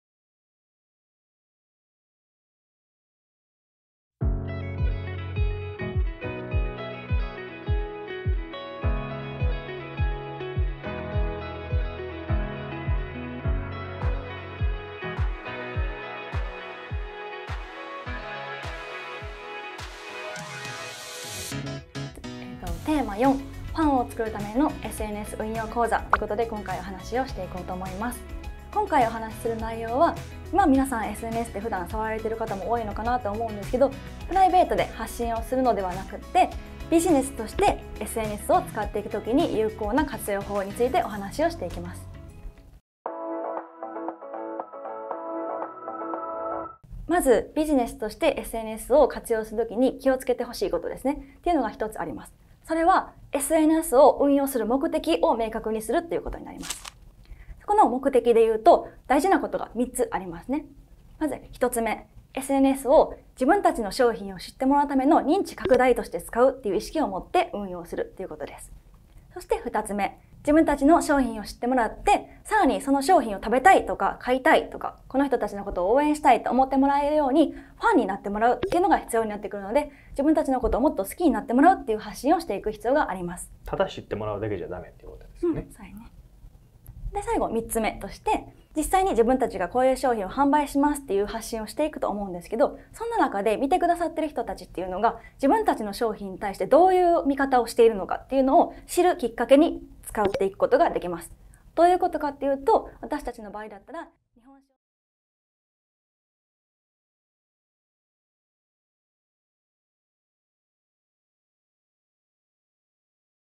【サンプル動画】chefnoセミナー動画_04『ファンをつくるためのSNS運用講座』
この動画は、chefnoセミナー動画_04『ファンをつくるためのSNS運用講座』のサンプル動画になります。

本編の動画は、以下URLよりご視聴ください。

・インターネット販売でパンやお菓子を販売したいと思われている方
・インターネット販売を始めるのに何から始めたら良いか分からない方
・販売や運営方法の質を高めたい方
・インターネット販売ご担当者様

❑講師からのメッセージ

」くらいのイメージでしたが、「なんとなく」の運用だとファンはできませんでした。SNSは「開始するタイミング」や「投稿する内容」すべてに意味があります。

「ファンを作る」という観点でお話しているので、いろんな人に応援されながら活動したいという方はぜひご覧くださいね。